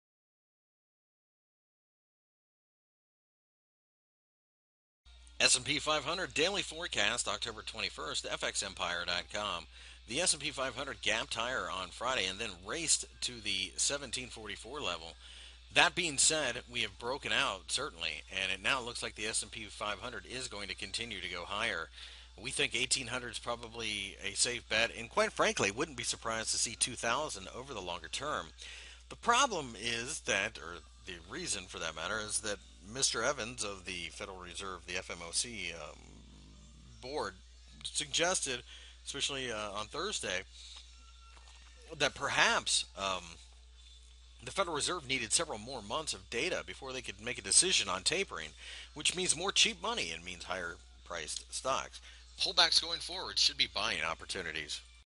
S & P 500 Technical Analysis for October 21, 2013 by FXEmpire.com
- October 21, 2013 index daily technical analysis for the S & P 500 INDEX. Find more information about Forex and Commodities News